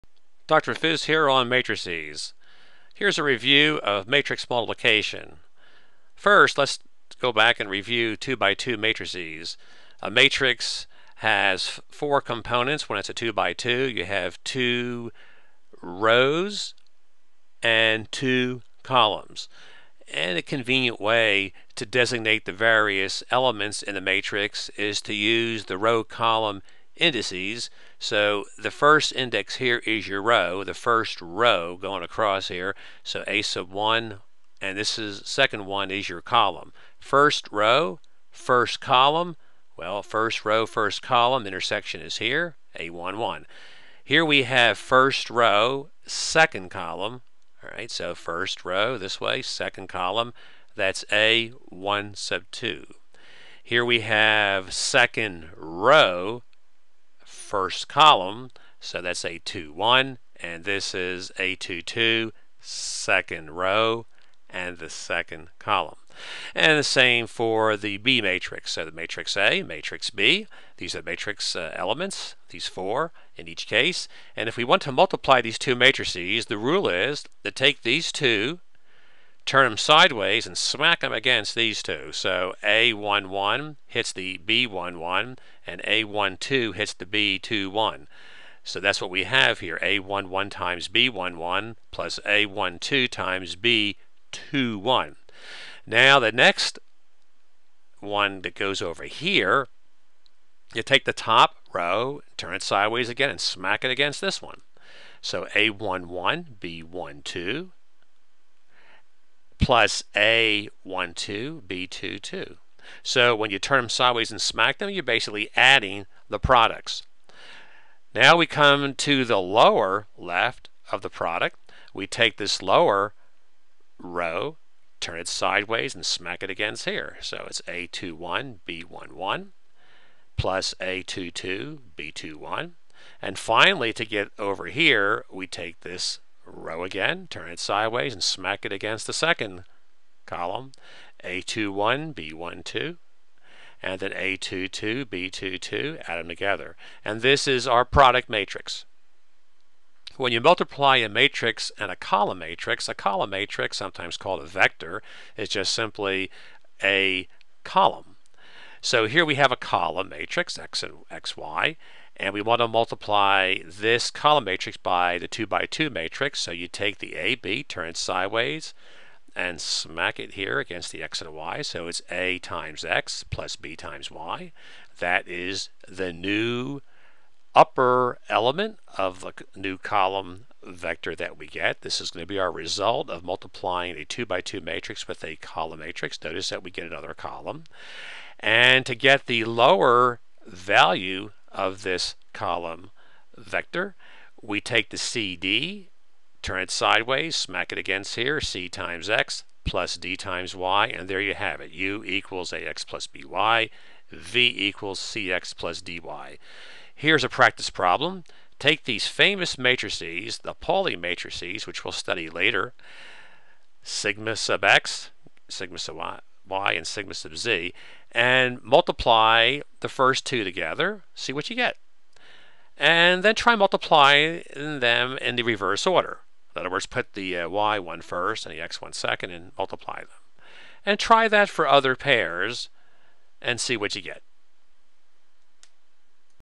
A3. Matrices
A short review of matrix multiplication.